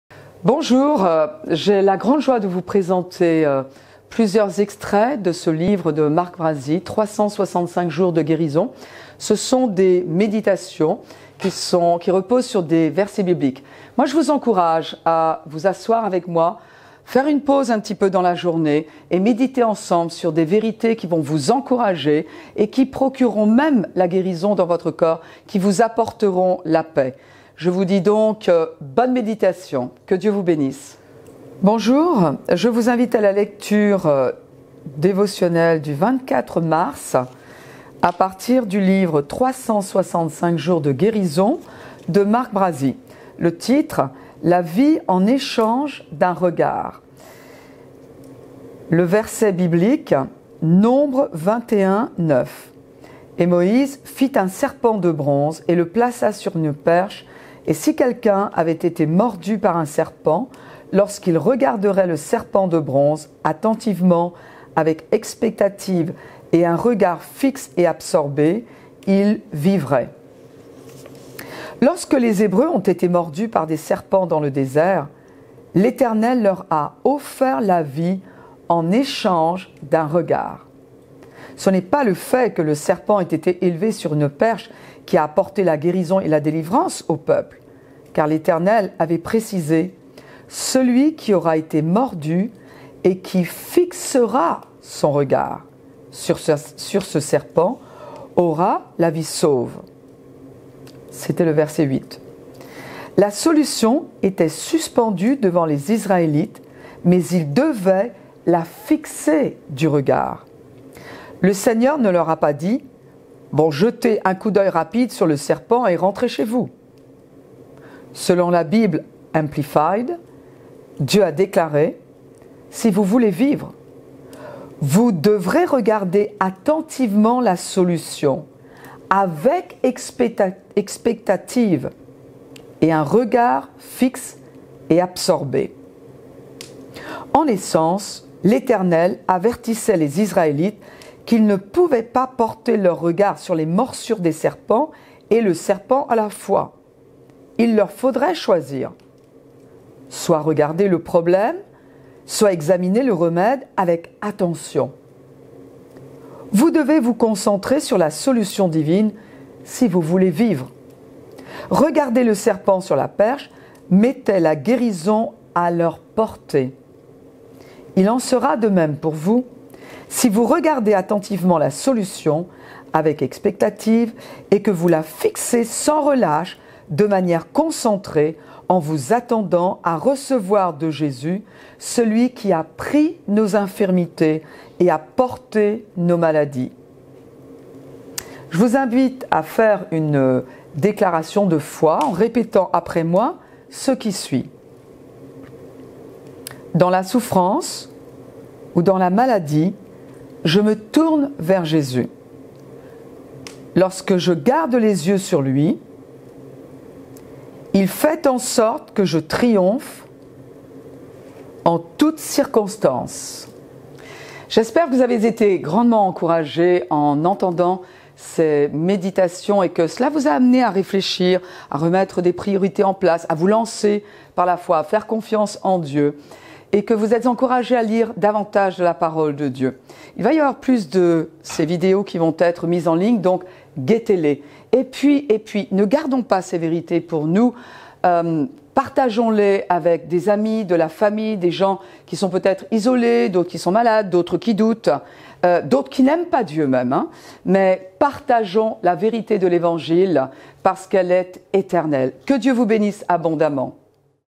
La vie en échange d'un regard (Épisode 3) - Marie-Hélène Moulin
Méditation guidée sur la guérison

Extrait issu du livre "365 jours de guérison" de Mark Brazee
Lu et animé par pasteur Marie-Hélène Moulin 🔥💙